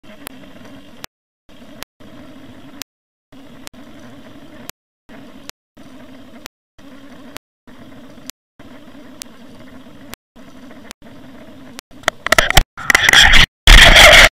Salmon Crushes Soturi Vixen
Staging King salmon nails a crankbait